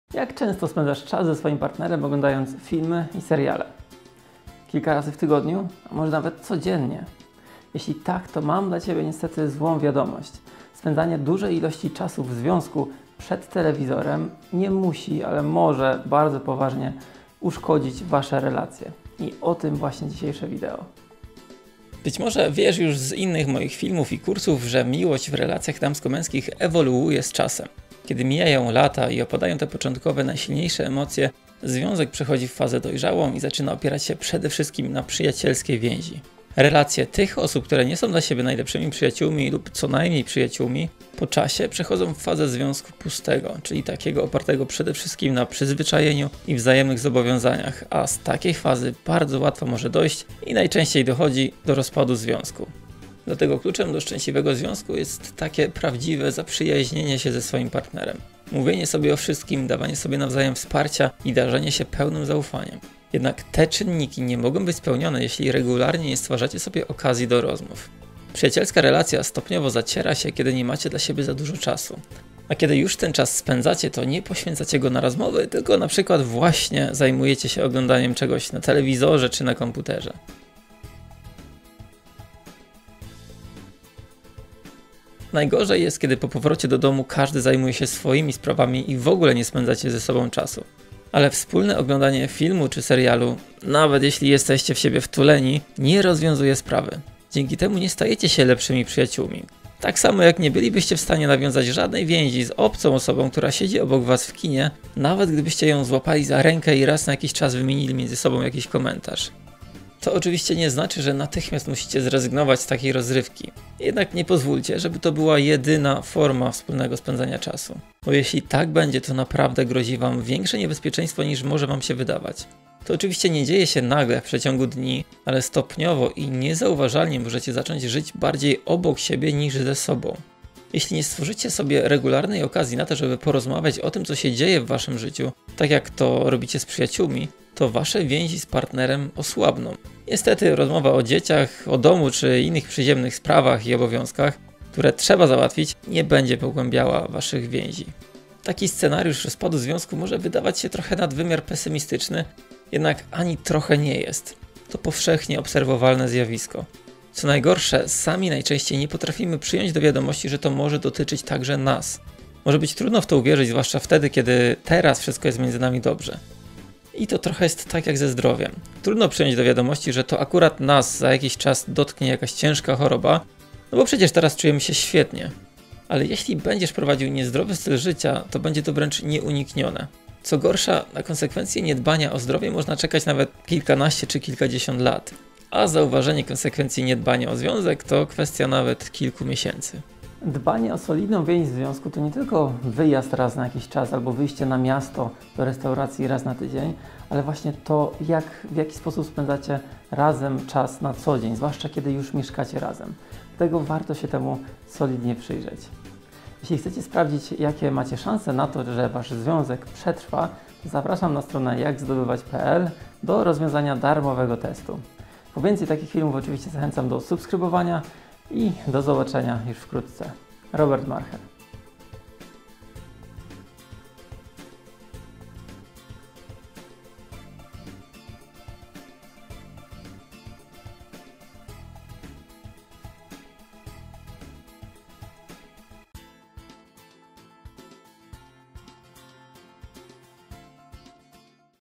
Czy seriale mogą zabijać Twój związek?
Czas wolny często spędzasz na oglądaniu seriali razem z partnerem? A czy zastanawiałeś się kiedykolwiek, czy może to wpłynąć na Twój związek? Jeśli nie - nie martw się, właśnie teraz będziesz miał okazję się temu przyjrzeć. Zobacz koniecznie, czy seriale nie zniszczą Twojej relacji.
⬇⬇⬇Znajdź mnie ⬇⬇⬇
Strona: